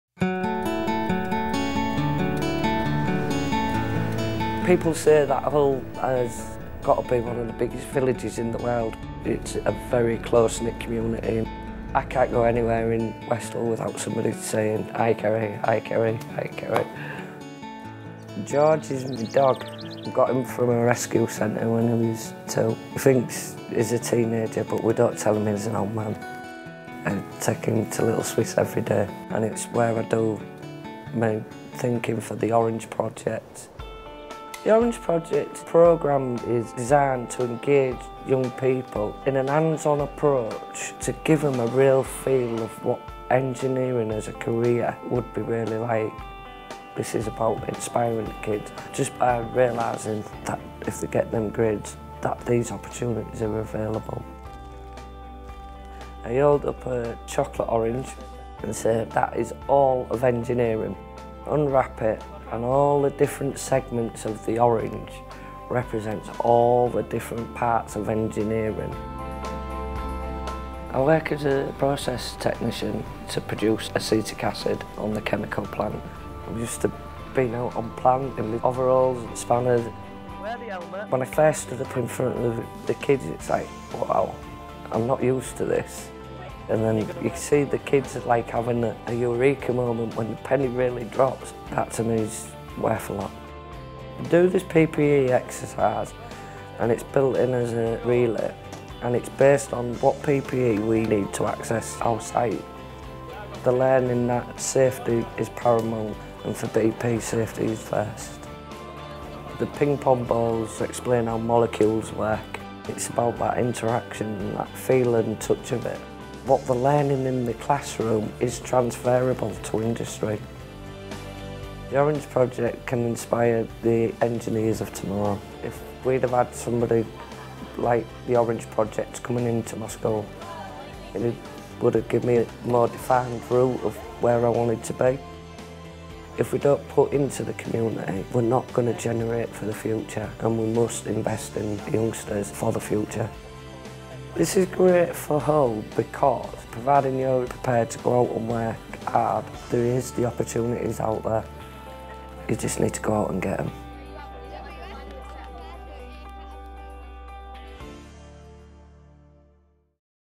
Meet Kerrie Hutchinson, BP’s process technician and school mentor in the UK
Kerrie Hutchinson gives back to the community she loves. From factory worker to working with young people, Kerrie Hutchinson’s career path lead her to start The Orange Project—an interactive presentation she takes to local schools in the hopes of inspiring future engineers.